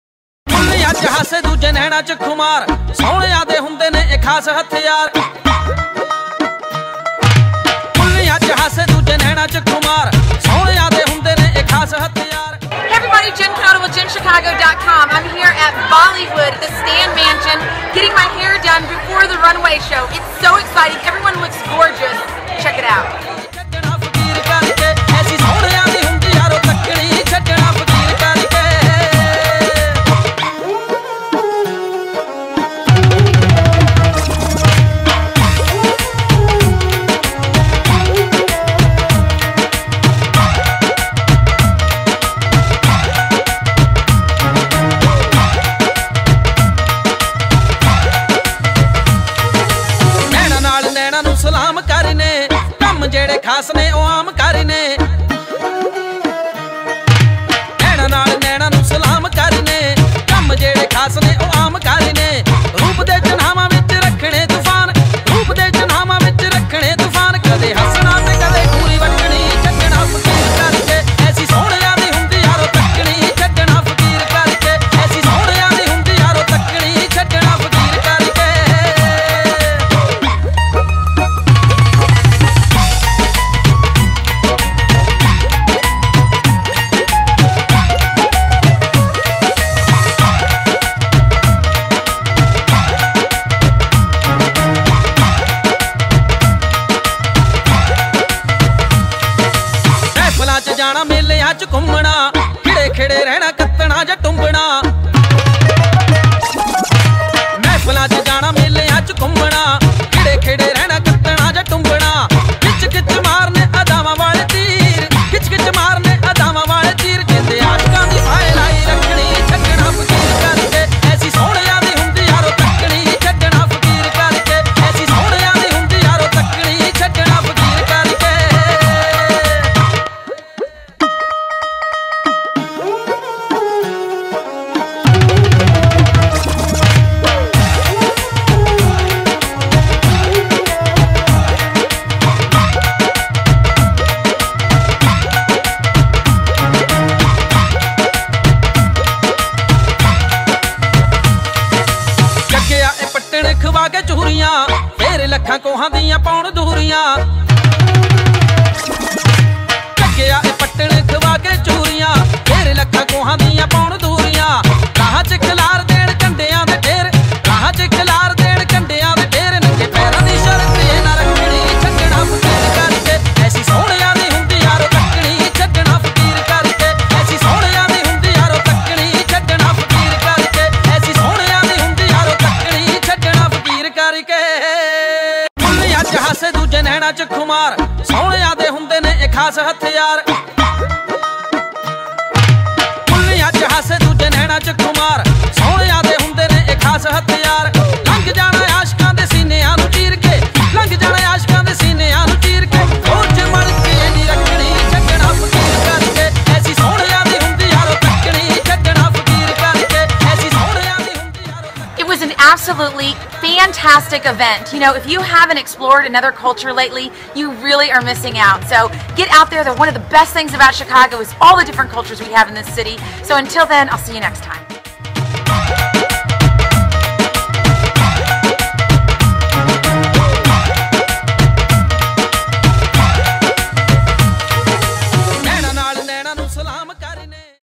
EVENT: Bollywood Blowout Fundraiser to Benefit @GiGisChicago with @JenTravelsLife (11-15-13)
Travel Vlogger and Cultural Enthusiast Jen Knoedl joins a star-studded crowd for Bollywood Blowout at Stan Mansion in Chicago.